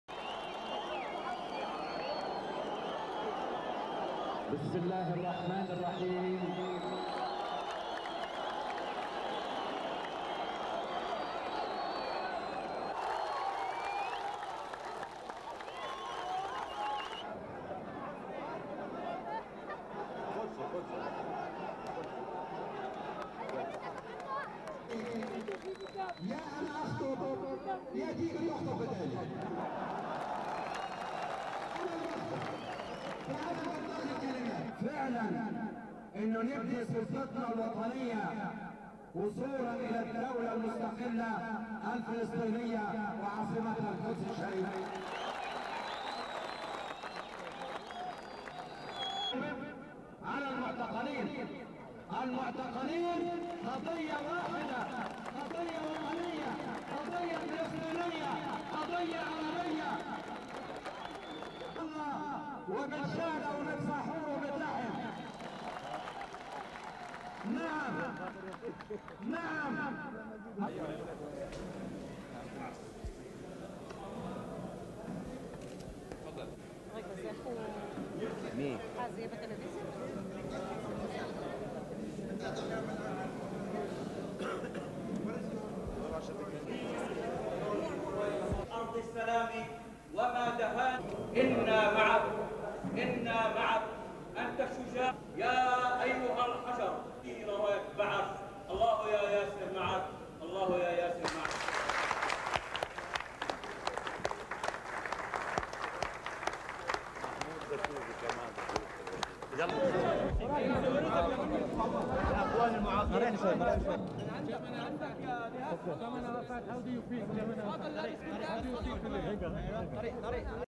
Israel - Anti-Arafat Demonstration
(2 Jul 1994) Thousands of Israelis chanting "Death to Arafat" and "Rabin is a traitor" packed into the heart of Jewish West Jerusalem on Saturday night (2/7) to protest at the return of PLO chief Yasser Arafat to Gaza.